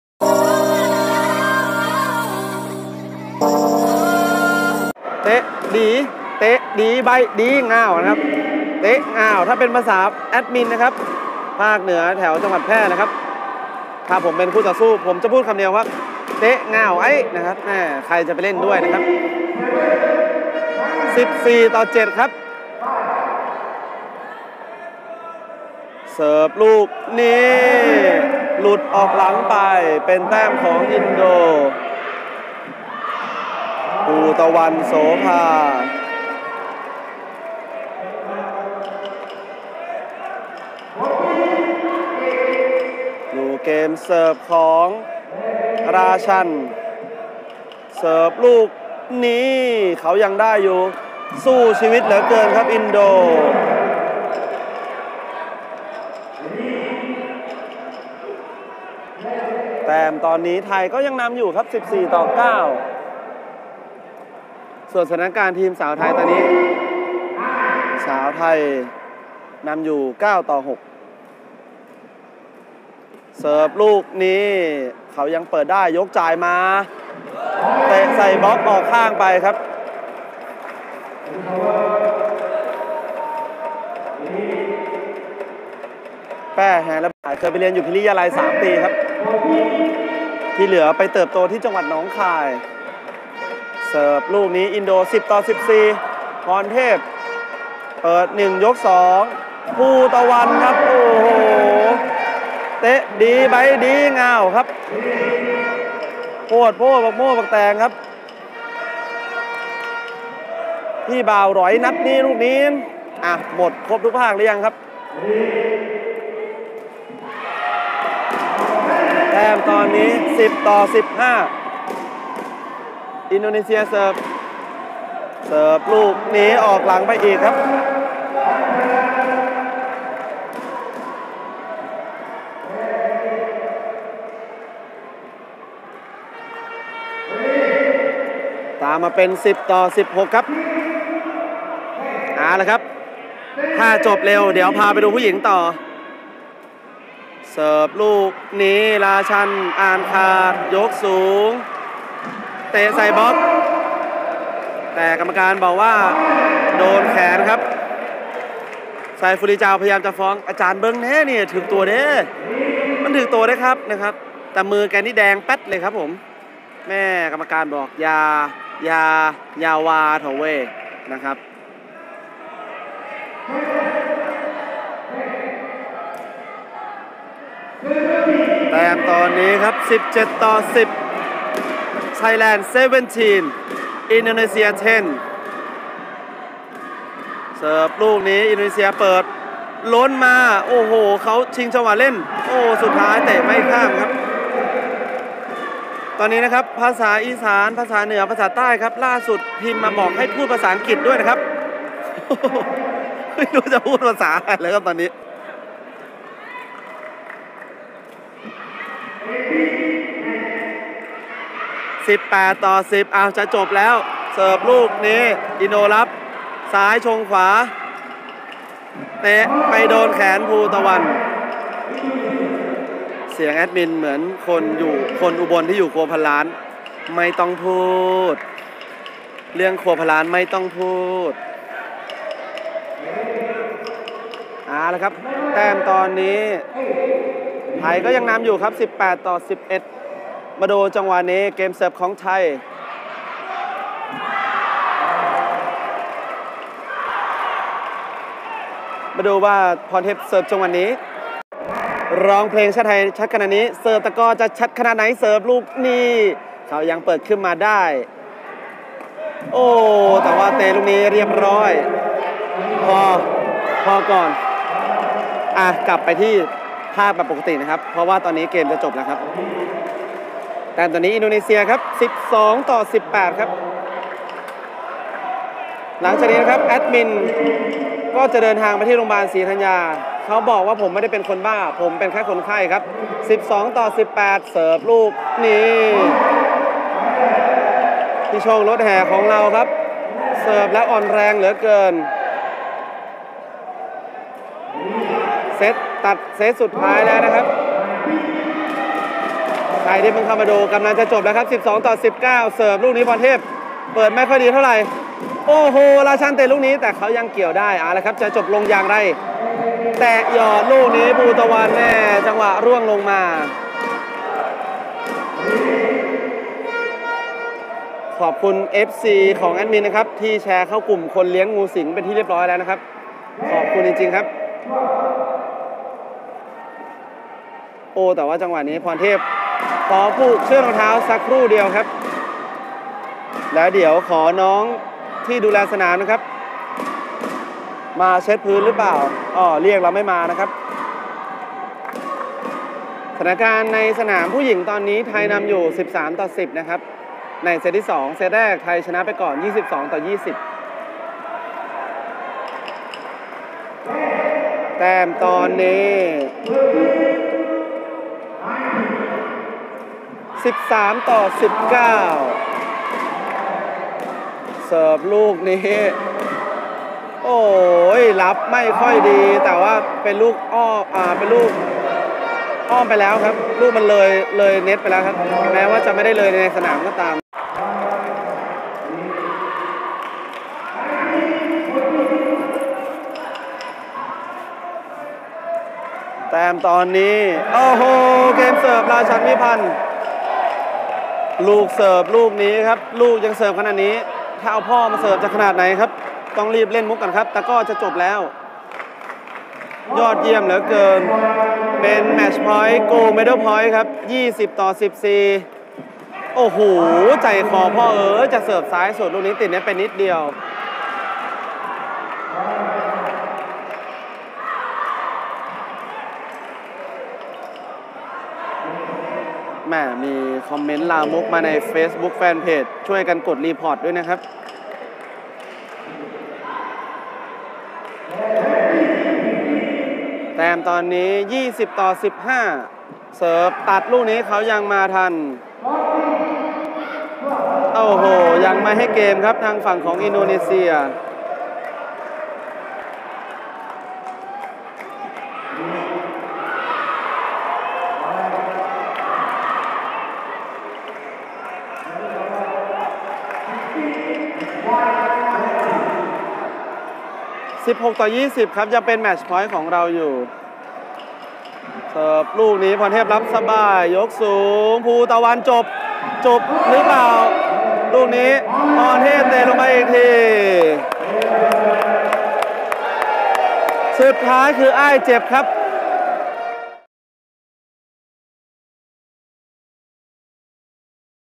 FINAL Indonesia 🇮🇩 vs 🇹🇭 Thailand Men's Qudrant Sea Games 2023 - Sepak Takraw 4 Pria
FINAL Indonesia 🇮🇩 vs 🇹🇭 Thailand Men's Qudrant Sea Games 2023